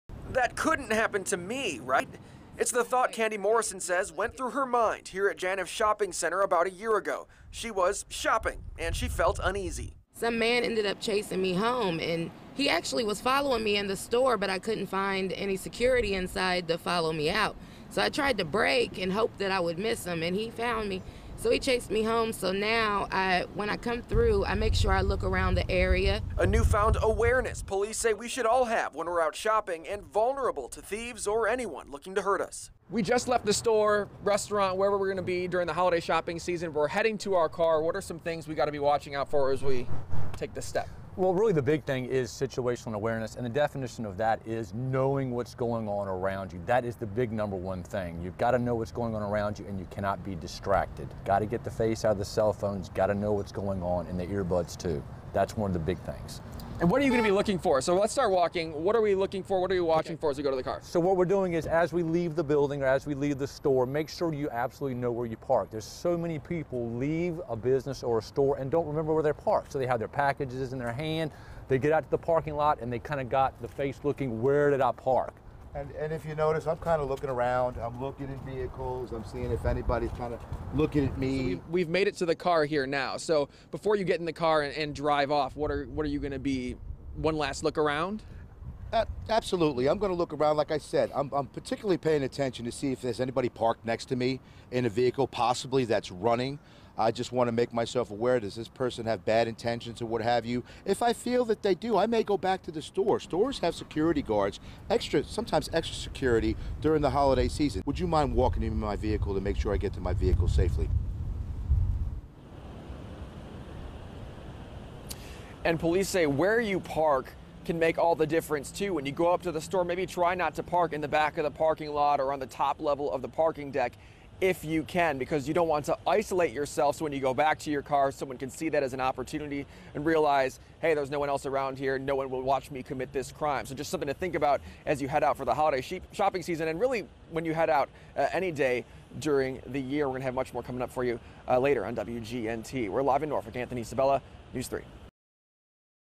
Shopping for the holidays? Police say stay aware of your surroundings
Picture this scenario: It's the holiday, it's dark and you're leaving the store with hands full of shopping bags as you head to your car. It's easy to get distracted and this is when police say people become vulnerable to someone looking to do harm.